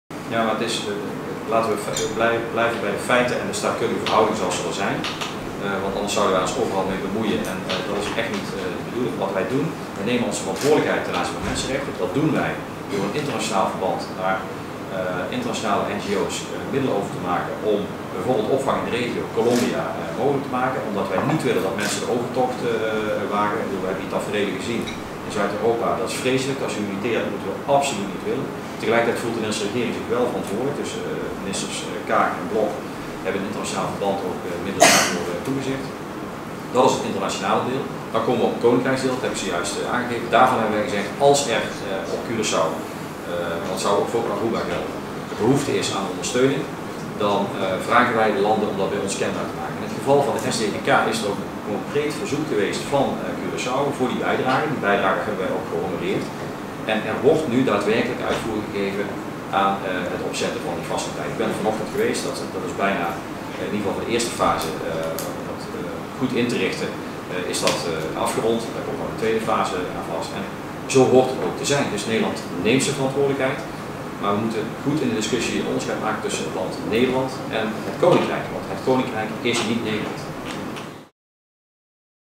Knops over asielbeleid Curaçao
Staatssecretaris Raymond Knops (Koninkrijksrelaties) zegt dat Curaçao een asielbeleid hanteert waarin onderscheid wordt gemaakt tussen economische migranten en mensen die recht hebben op asiel en dat Nederland zich daarmee niet hoeft te bemoeien. Dat heeft Knops woensdag geantwoord op vragen tijdens een persconferentie op Curaçao. Door Kim Hendriksen.